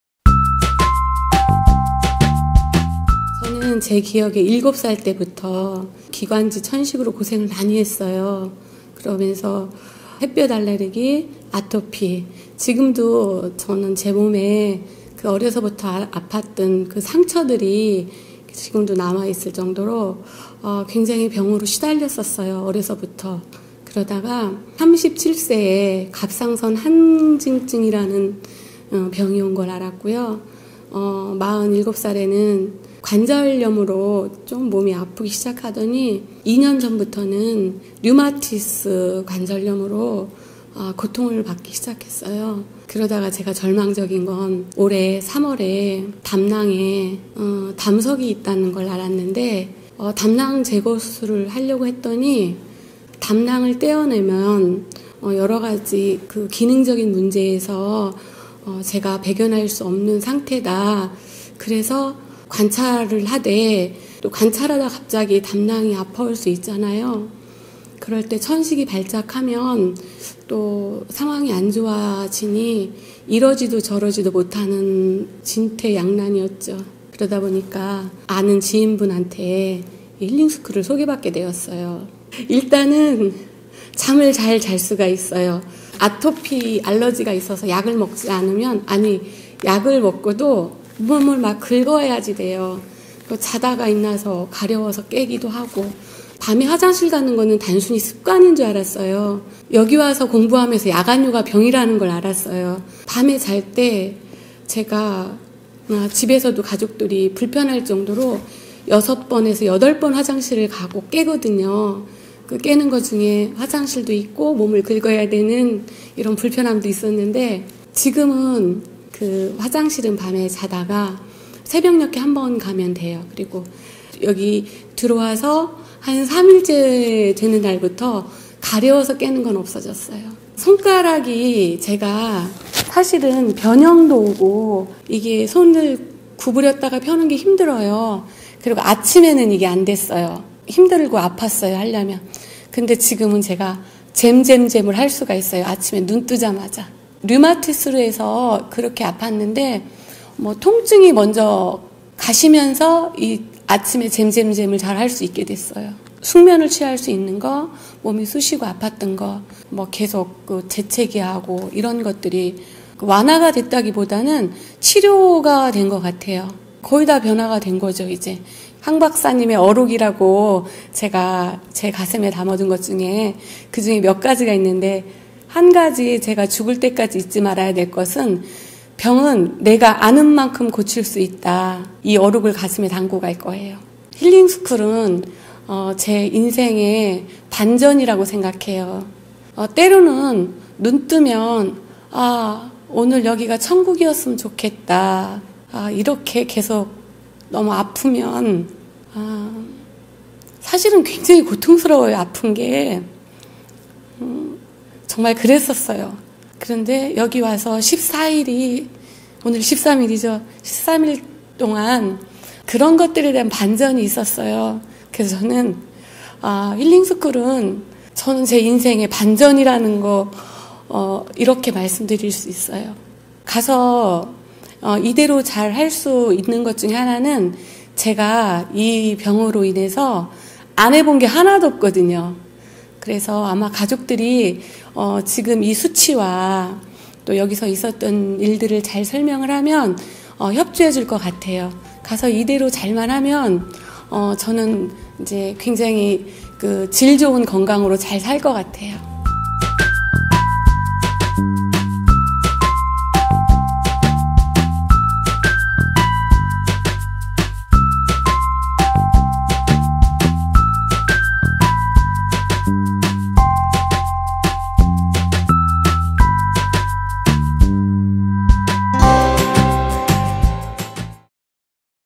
[ENG SUB] 류마티스 관절염, '힐링스쿨은 제 인생의 반전이라고 생각해요'
내 인생에 반전을 가져다준 몸의 변화
병은 내가 아는 만큼 고칠 수 있다는 황 박사님의 말씀을 가슴에 담고 가겠습니다.
자연식물식 황성수 박사의 고혈압, 당뇨, 비만, 만성신부전증 탈출학교 '황성수힐링스쿨'  자연식물식으로 건강을 되찾는 비결을 소개합니다.

* 자연식물식에 대한 질문은 아래 재생목록으로 정리해두었으니  참고해주세요.